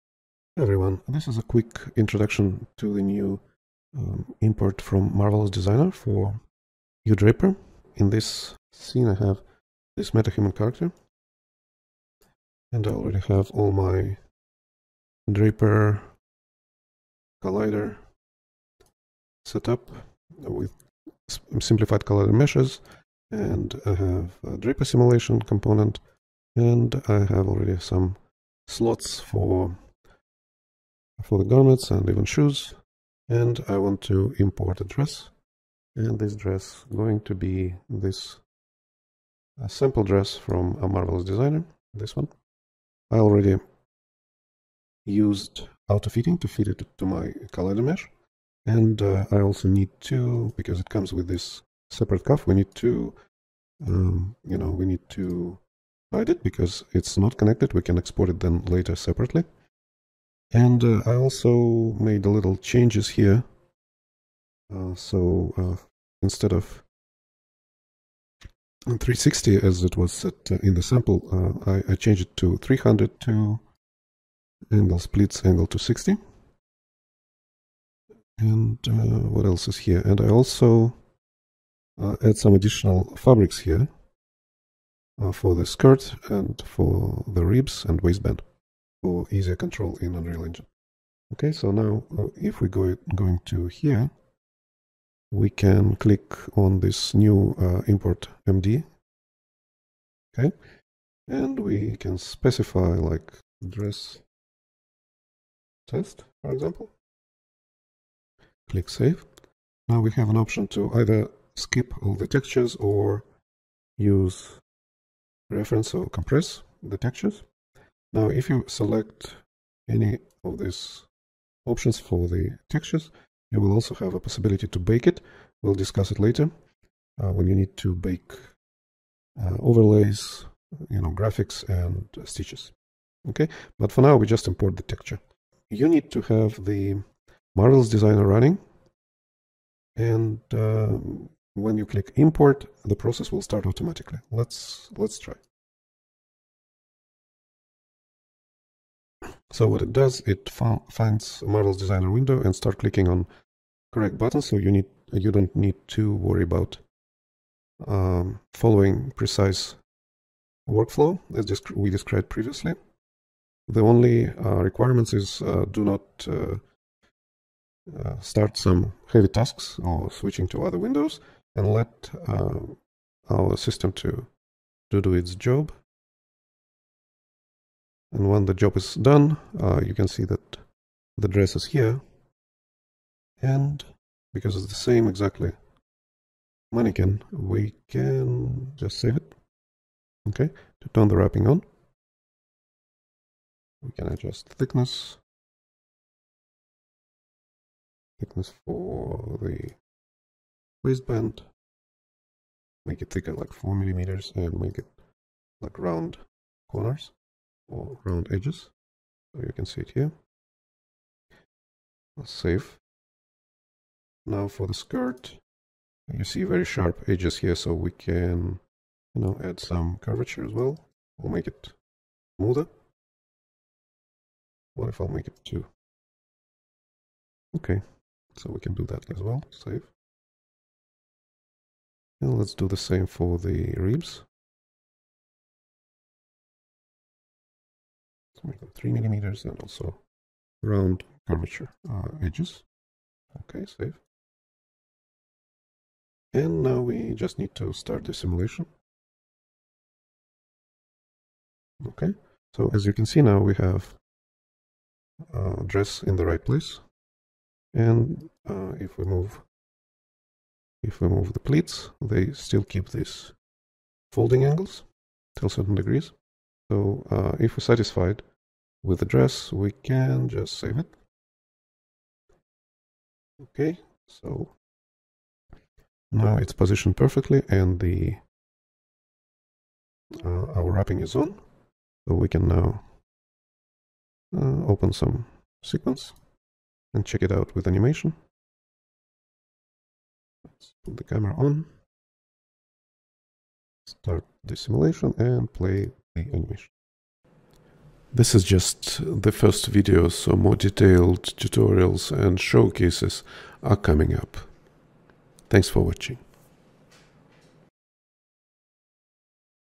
Marvelous Designer Clothes to Unreal Engine for Real-Time Cloth Simulation - Major Update
This is the first video about the latest major uDraper update that makes easier to import clothes from Marvelous Designer to Unreal Engine by automating the workflow. In this video we show just the basic functionality. More detailed tutorials and showcases are coming soon.

In this update we've implemented the following enhancements for importing from Marvelous Designer:
* Automatic import
* Layers
* Shrinkage (grain angle must be 0)
* Folds on internal lines
* Folds on seams (conditionally)
* Flat shading on seams (conditionally)
* PBR textures (conditionally).